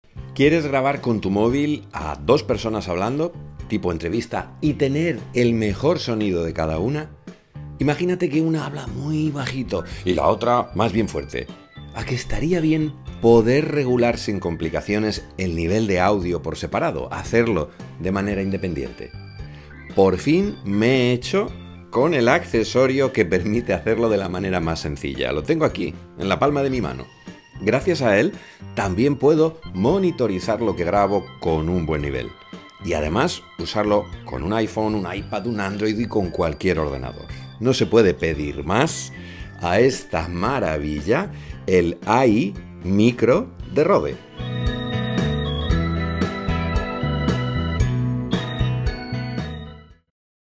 Cómo conectar tu Micro a cualquier dispositivo
🎙 Es posible que tengas un micrófono que funciona en tu móvil Android, pero no hay manera de conectarlo a un iPad o a tu ordenador para grabar vídeos o hacer un Zoom, y que se te escuche con claridad.

¿Estás pensando en comprarte otro micrófono?🤔

👀 Mira primero el accesorio que te presento hoy.

👉 Es el AI Micro de Rode, un adaptador con el que vas a conectar cualquier micrófono que tenga un conector minijack a un iPhone, iPad Pro, Android, PC o Mac… ¡casi nada!

Vas a poder hacer eso y muchas cosas más que te cuento en la review en Youtube.😉

🧐 ¿Qué te parece este accesorio para grabar tus vídeos como un pro?